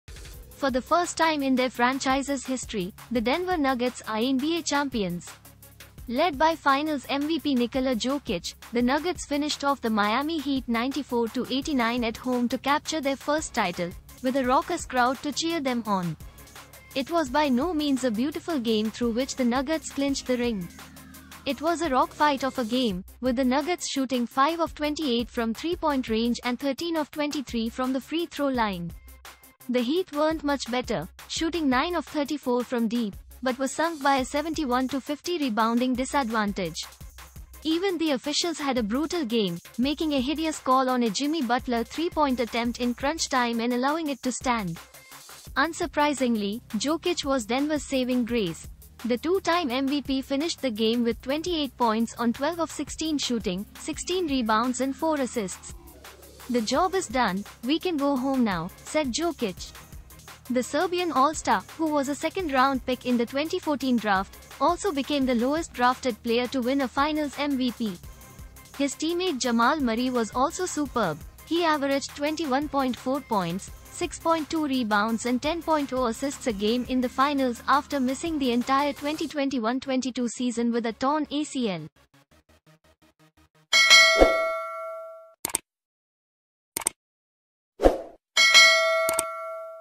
Denver Nuggets are NBA Champions
The Denver Nuggets clinched their first NBA title as a 94-89 win over the Miami Heat sealed a 4-1 series triumph.